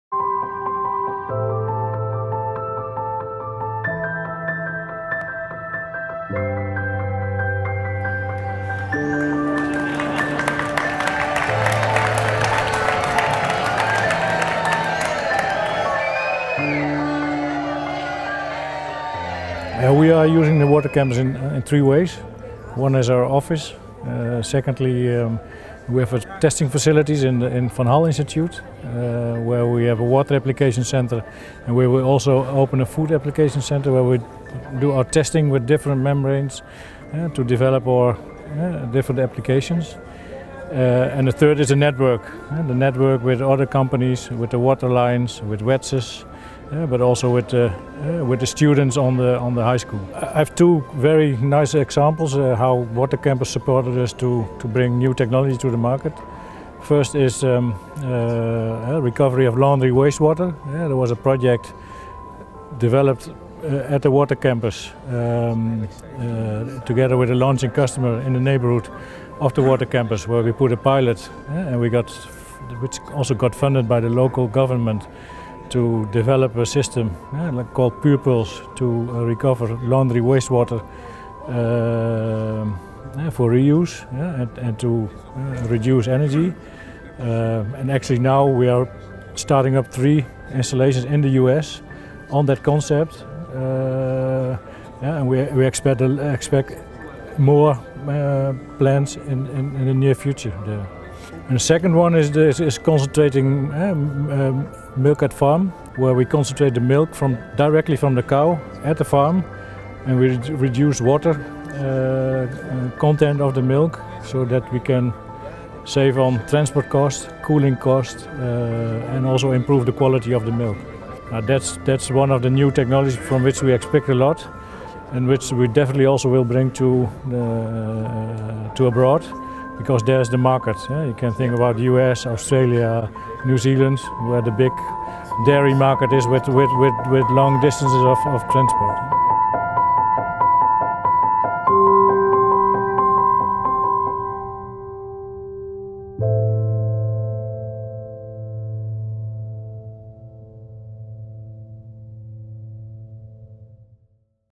CEO Henk Schonewille Wafilin Systems at EWTW2018
'Concentrating milk at the farm is saving transport- and cooling costs and improving the quality of the milk'! At the EWTW2018, Tom Freyberg interviewed CEO Henk Schonewille about the impact of new innovations. Check out the full interview.